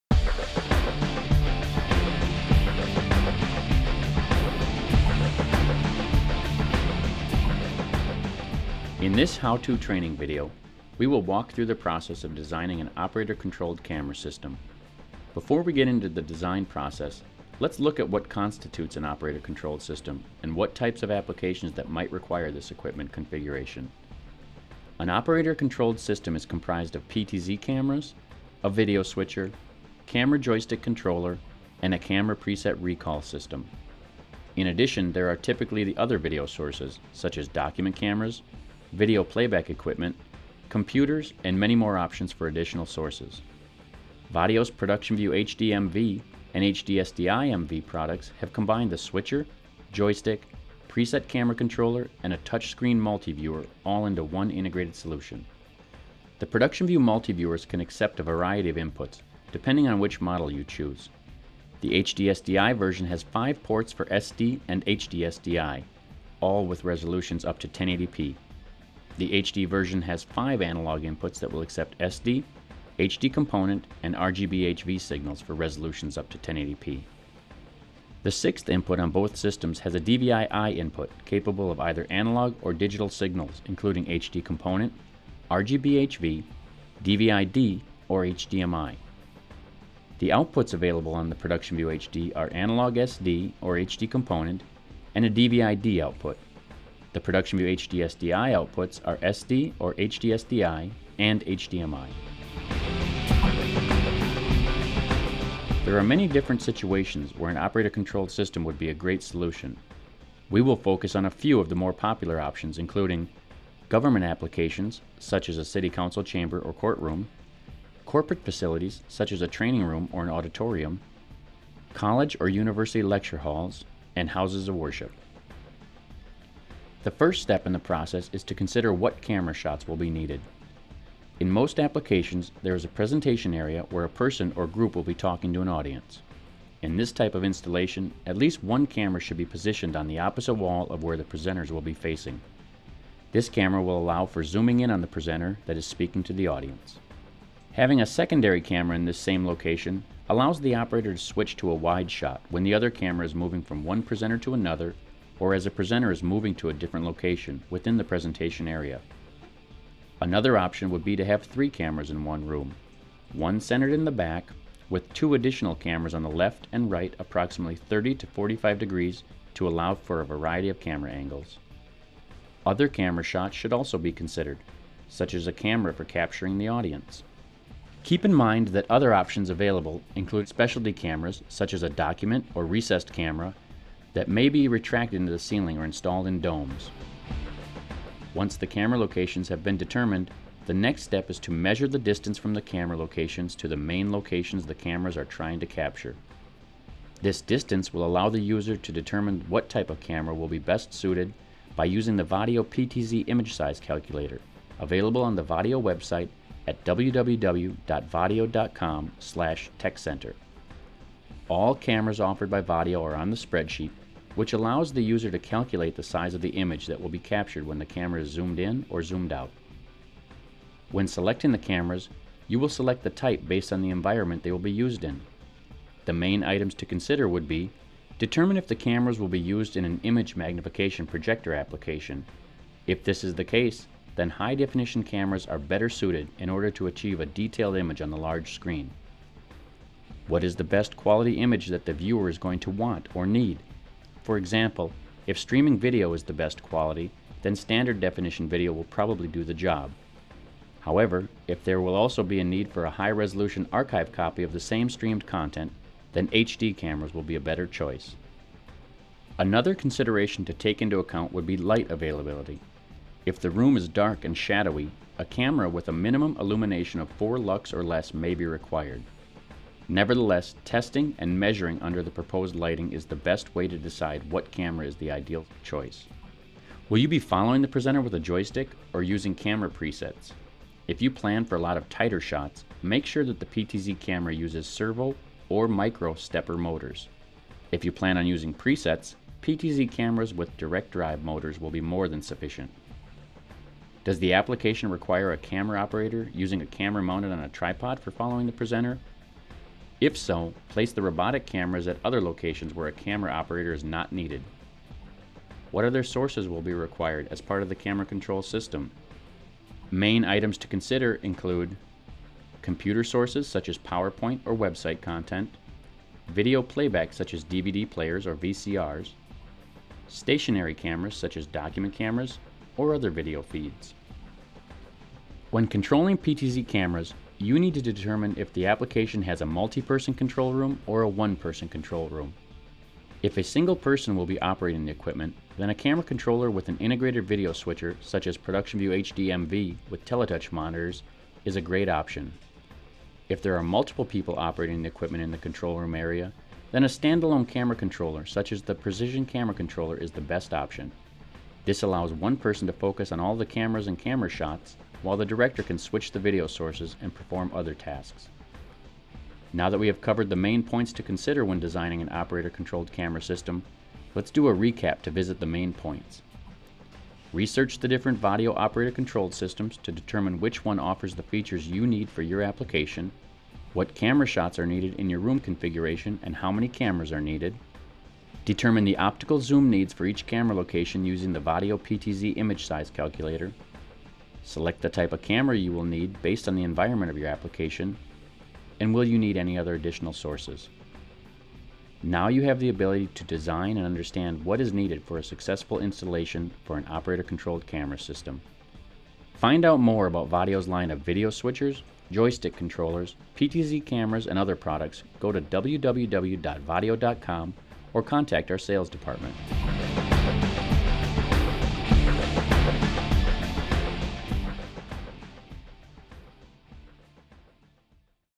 How to Design an Operator Controlled Camera System
In this how-to training video, we will go through the types of equipment that are needed to create an operator controlled system. These include PTZ cameras, video switcher, camera joystick controller, and a camera preset recall system. In a future video, we’ll go over the design elements for an operator controlled system.